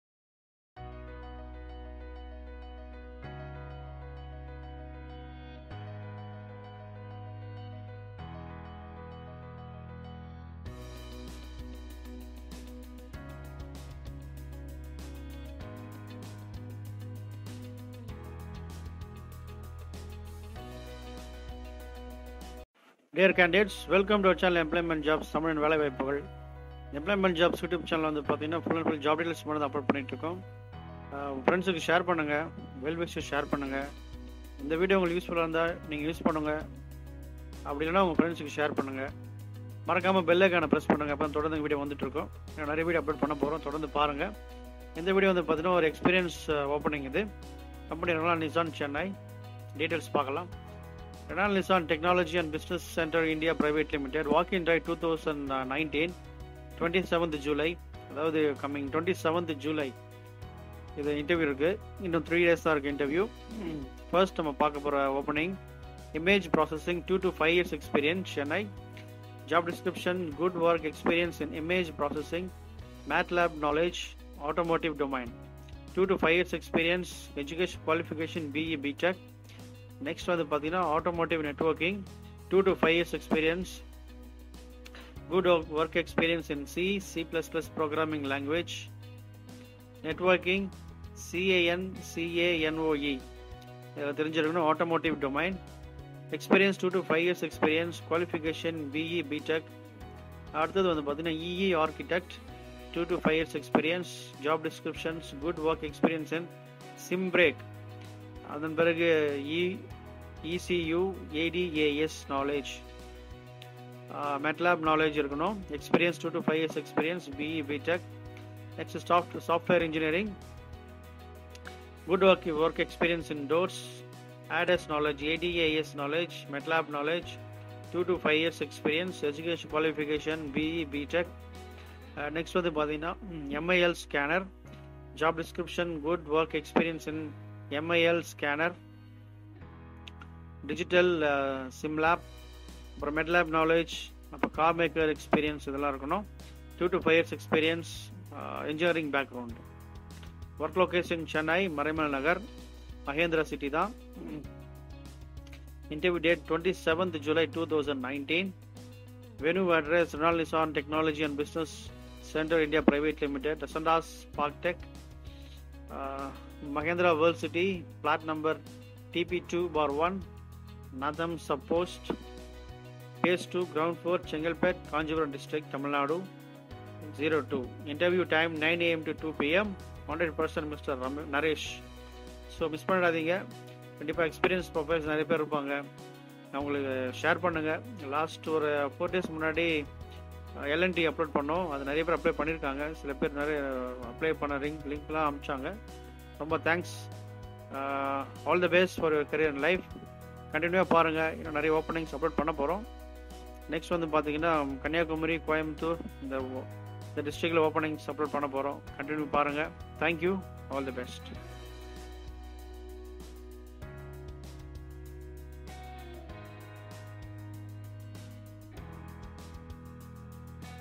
Nissan Walk in Interview - 27/07/2019. Chennai
RENAULT NISSAN TECHNOLOGY & BUSINESS CENTRE INDIA PRIVATE LIMITED
Walk in Drive in Chennai   27th July 19
Image Processing
2 - 5 yrs
Chennai
Job Description :
Good Work experience in Image Processing
Matlab Knowledge
Automotive Domain
Experience : 2 to 5 YEARS
Educational Qualifications :
BE/ B TECH

Automotive Networking
2 - 5 yrs
Chennai
Job Description :
Good Work experience in C & C++ Programing Language
 Networking - CAN CANOE,
Automotive Domain
Educational Qualifications :
BE/ B TECH

EE Architect
2 - 5 yrs
Chennai
Job Description :
Good Work experience in SIM BREAK
 ECU / ADAS Knowledge
 Matlab Knowledge
Educational Qualifications :
BE/ B TECH

Software Engineer
Job Description :
Good Work experience in doors
 ADAS Knowledge
 Matlab Knowledge
Educational Qualifications :
BE/ B TECH

MIL Scaner
Job Description :
Good Work experience in MIL SCANER
 Digital Simlab.
Matlab Knowledge
Carmaker Experience
Educational Qualifications :
Engineering Background

Work Location : Chennai
Interview Date : 27th July 2019

Venue Address:
Renault-Nissan Technology and Business Centre India Private Ltd
Ascendas Tech Park, Mahindra World City SEZ
Plot No TP2/1, Natham Sub Post Office
Phase II, Ground Floor, Chengalpet, Kancheepuram District
Tamilnadu 603002

Interview Time : 9.00 AM to 2.00 PM
Contact Person : Naresh